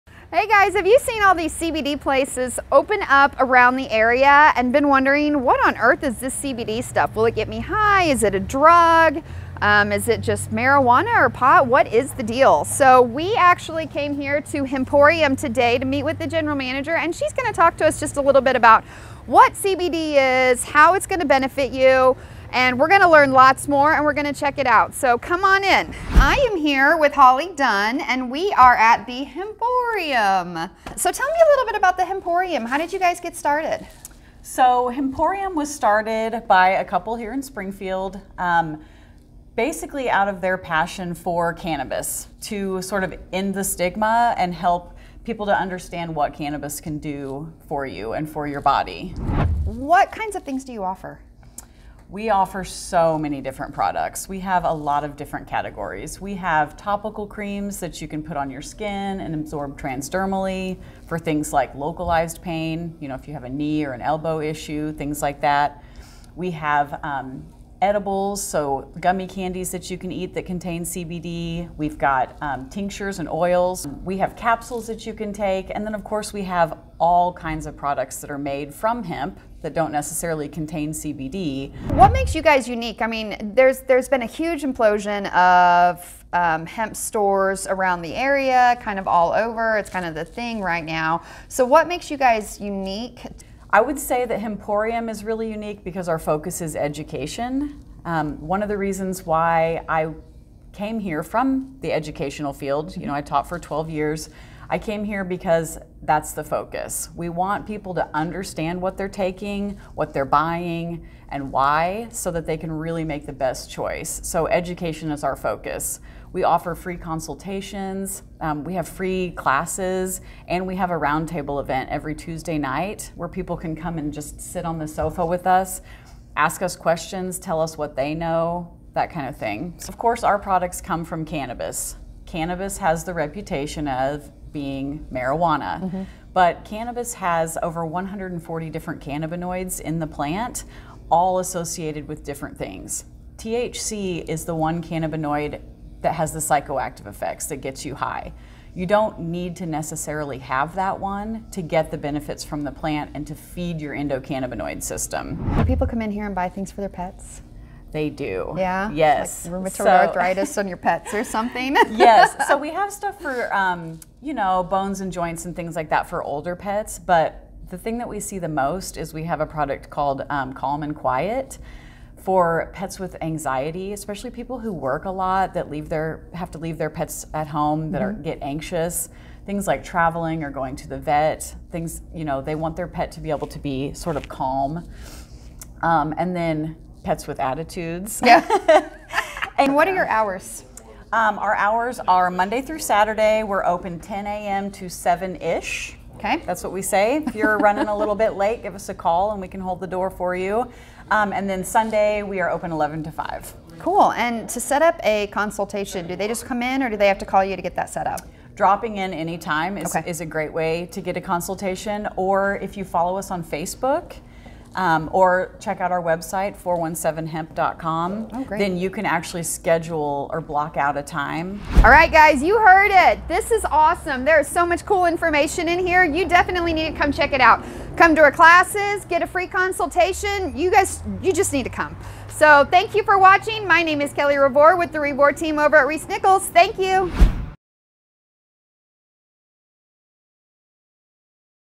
Springfield Living: What is CBD? // Hemporium in Springfield // The Healing Powers of Cannabis
Recently, towns all over have been popping up with dispensaries displaying products like CBD and edibles. If you’re like me you’re wondering what the heck is CBD and what are these shops contributing to our communities? There is a stigma around them that I was not ready to believe in, so I decided to head over to Hemporium at 4139 S. National Avenue and find out for myself.

I spoke to Holly, a professional at Hemporium, who used to teach for twelve years. Holly made the switch because she is passionate about the healing power cannabis has and wanted to mix her profession in with her passion to inform and introduce others to the benefits as well. This aspect is something that makes Hemporium different from all the other dispensaries out there; they are committed to educating their community about all things cannabis related. They offer free consultations to ensure every question you have is answered and that you get the proper product for your needs and your budget. Classes and table talks are also offered at Hemporium on a weekly basis. To find out more about any of those, you can check out their facebook, pop into the shop, or visit their

After speaking with Holly, I can say that the stigma couldn’t be more wrong. She went into depth about all of the different parts of cannabis and that the psycho active aspect of it is referred to as THC. Majority of the products they offer in their shop does not contain this component and it is very easy to get the treatment you need without getting high. Their boutique is filled with all different sorts of products from edibles, cartridges, oils, pills, and topical lotions. There is truly something there for anyone to benefit from, even your furry friends! That’s right, they offer products for your pets too! Whether you have a frosted face with arthritis, or an animal with anxiety or an attitude, they’ve got you covered.

If you are at all interested in learning about the benefits of cannabis I definitely recommend seeing for yourself and going into the shop. All of the professionals that work there are highly educated on all their products and can match you with exactly what you need to be the best version of yourself, whether you need something for pain, anxiety, migraines, they’ve got it all. Many of their products are Gluten Free, Vegan, and 100% organic and are open Mon-Sat: 10am-7ish and Sun: 11am-5pm.